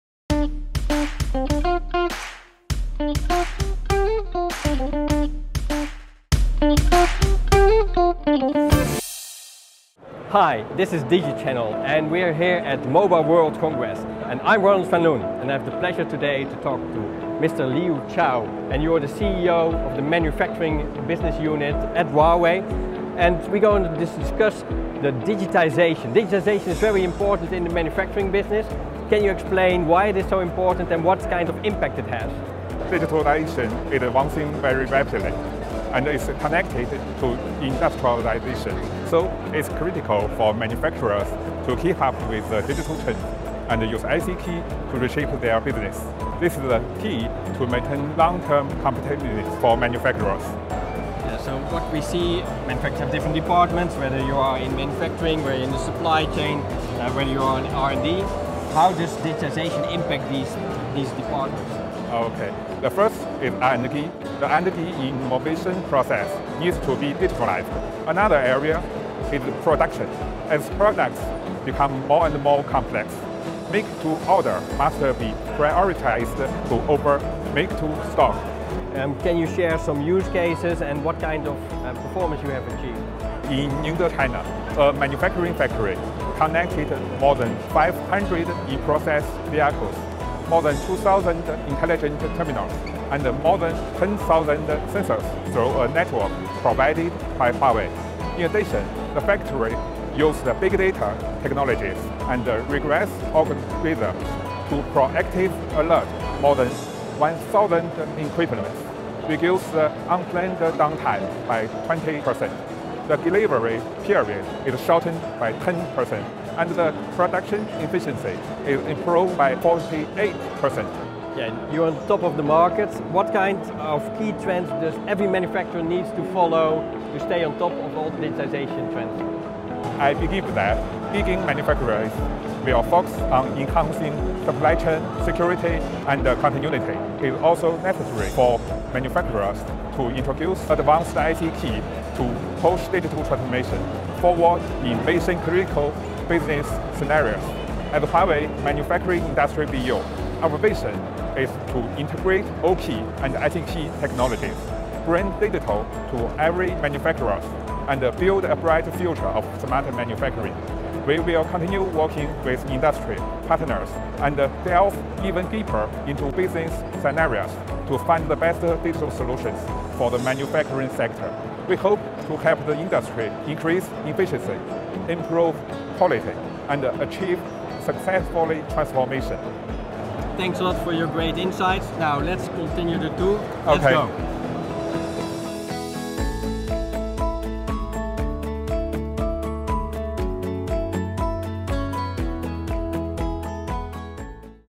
DIGI-Channel: The Latest Solutions and Practices in the Manufacturing Industry
Huawei's Liu Chao and Ronald van Loon discussed the latest solutions and practices in the manufacturing industry that will bring the digital world into every manufacturing enterprise and create a bright future for smart manufacturing.

KOL Visit Exhibition: Liu Chao and Ronald van Loon